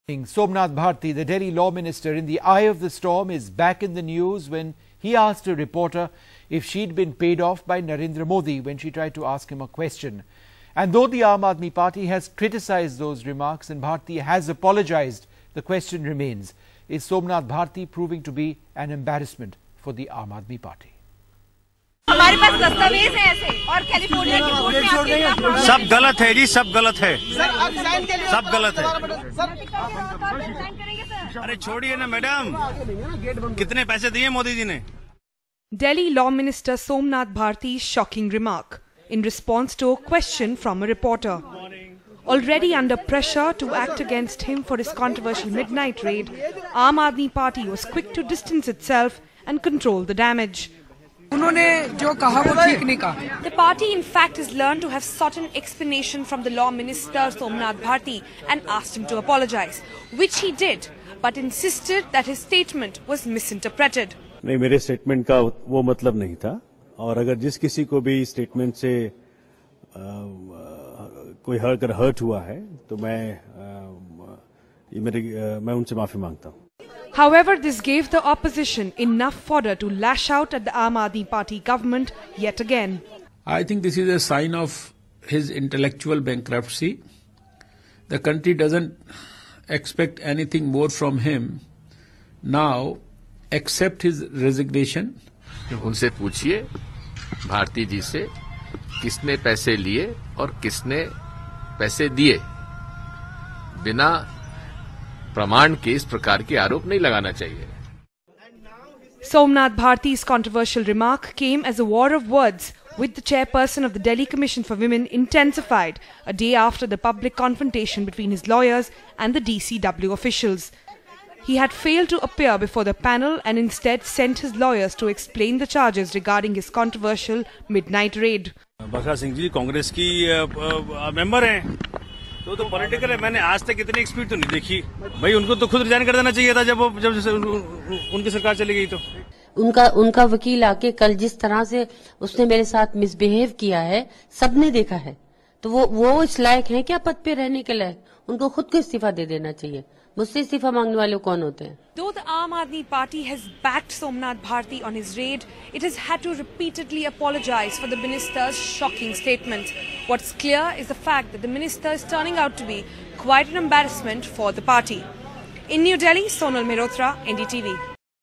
After accusing reporters of taking money from Modi, Somnath Bharti apologises
Amid a growing controversy over his "midnight raid" and demands for his resignation, Delhi Law Minister Somanth Bharti today accused the media of taking money from BJP leaders to defame him. When asked if he would resign on moral grounds, he even asked a reporter, "How much money have you got from (Narendra) Modi?" The minister later apologised but added that his statement "was being misinterpreted." Chief Minister and Aam Aadmi Party chief Arvind Kejriwal said his comments were "inappropriate."